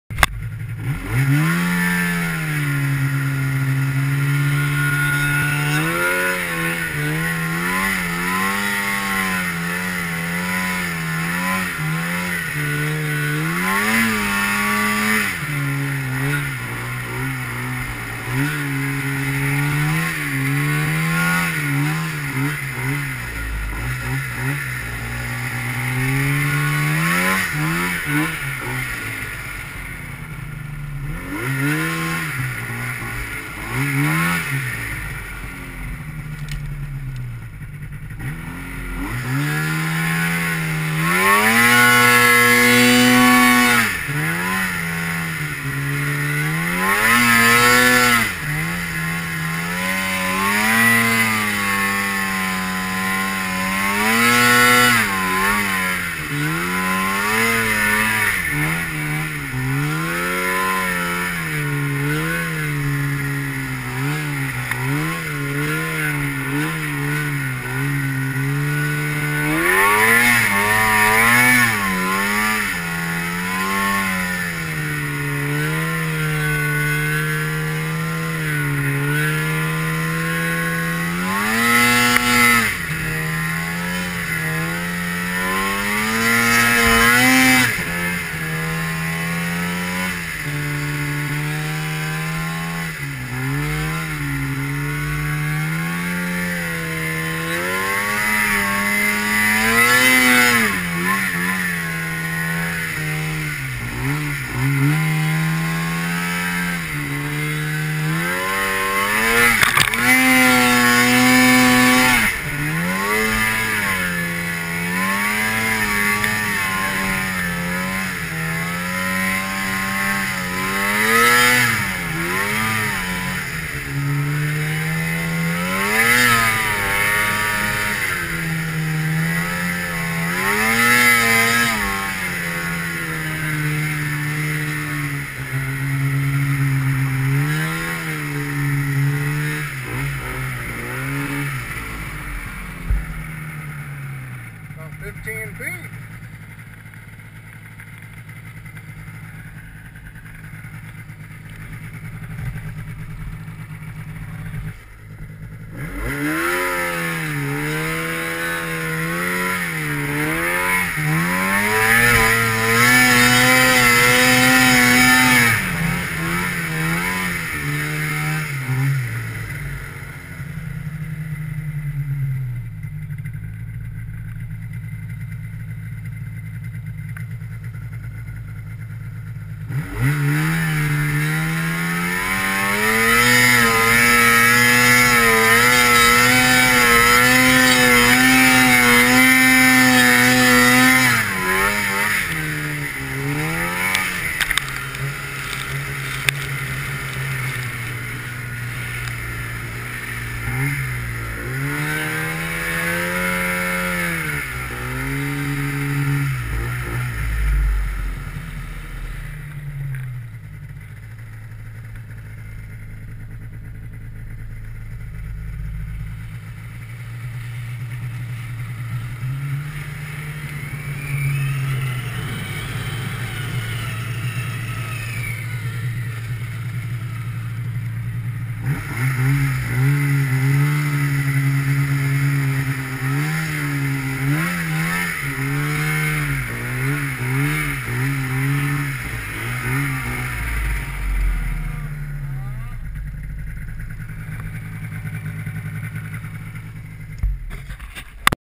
15' in the trees at Pinegrass 4-16-11
Making my way into the hill climb.